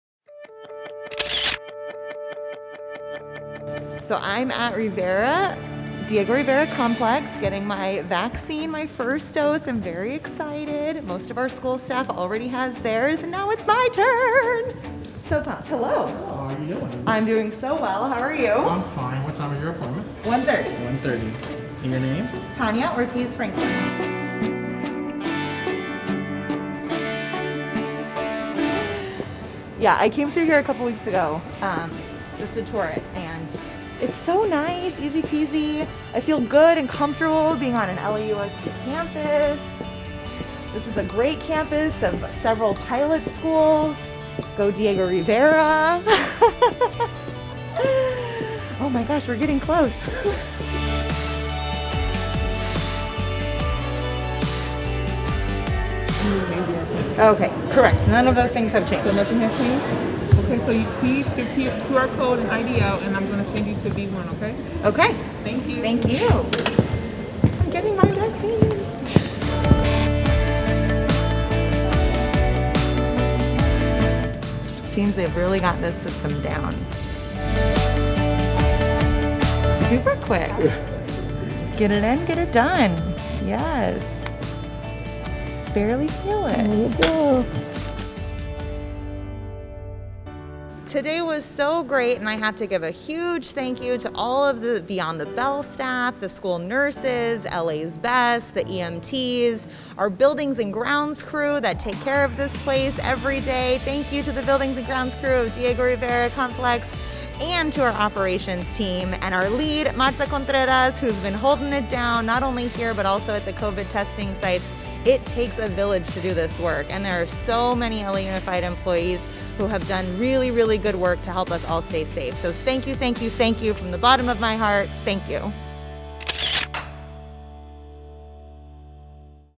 Diego Rivera Learning Complex Vaccination Site Visit
On March 25, Board Member Ortiz Franklin celebrated her 100th day in office by receiving her first vaccine dose at the Diego Rivera Learning Complex vaccination site. We would like to express our gratitude to all the LA Unified nurses, Beyond the Bell, LA’s BEST, operations team members, and emergency medical technicians for your hard work and commitment to ensuring our communities stay safe and healthy.